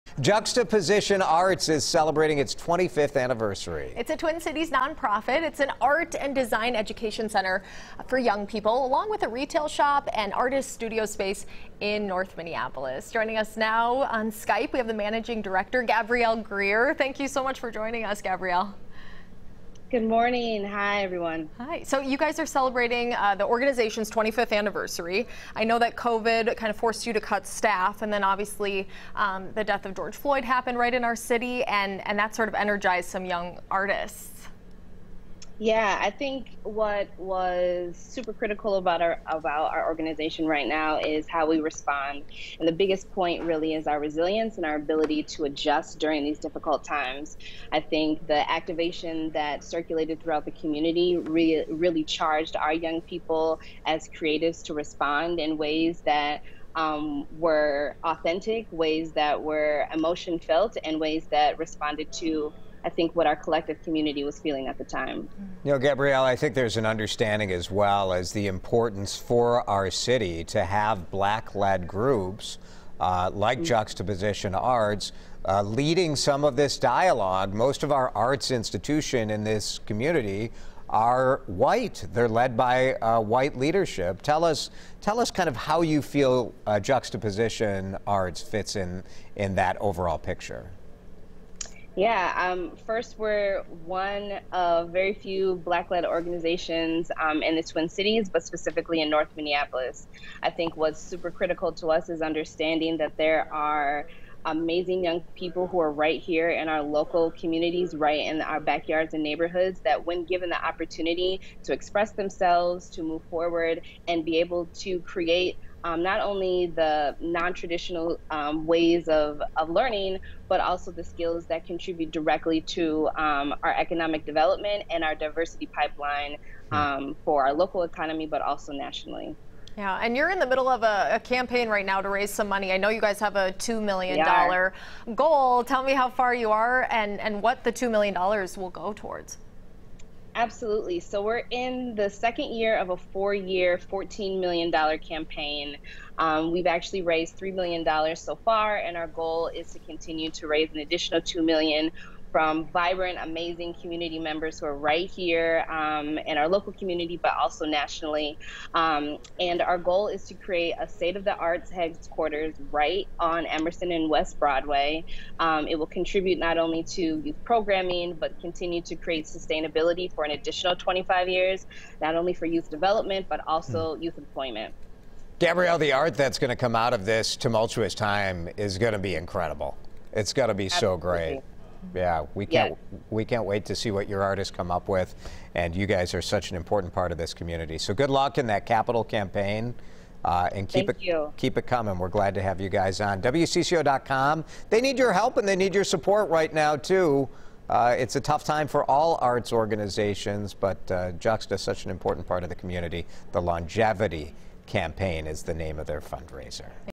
Juxtaposition Arts Celebrates 25 Years
The Twin Cities nonprofit is an art and design education center for youth, along with a retail shop and artists' studio space in North Minneapolis, managing director Gabrielle Grier explains (3:26). WCCO Mid-Morning - October 6, 2020